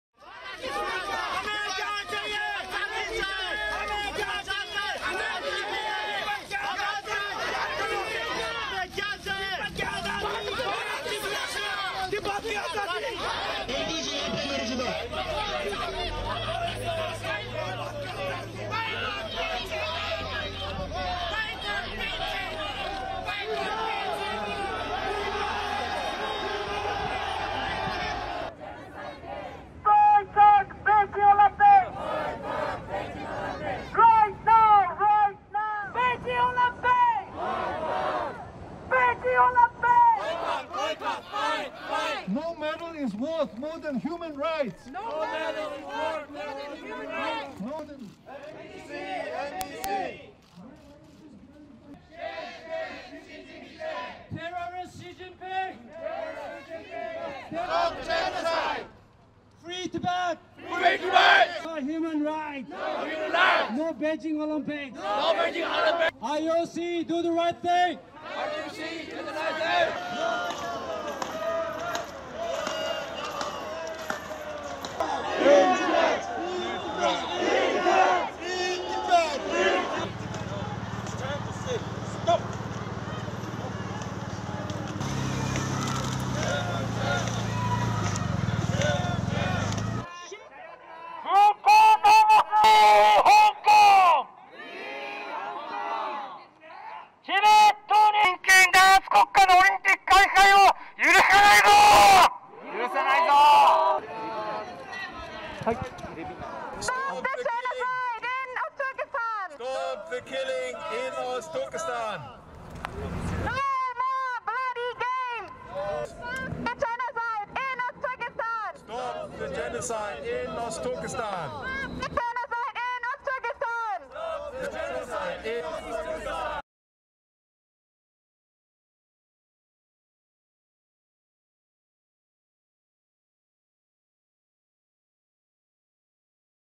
Protests around the world against the 2022 Beijing Winter Olympics- News 360 Tv
The 2022 Beijing Winter Olympics kicks off with a massive opening ceremony.  This comes amid widespread international protests concerning China's human rights record. Activists protested against the human rights abuses of the Chinese Communist Party and called for a boycott of the 2022 Olympic Games.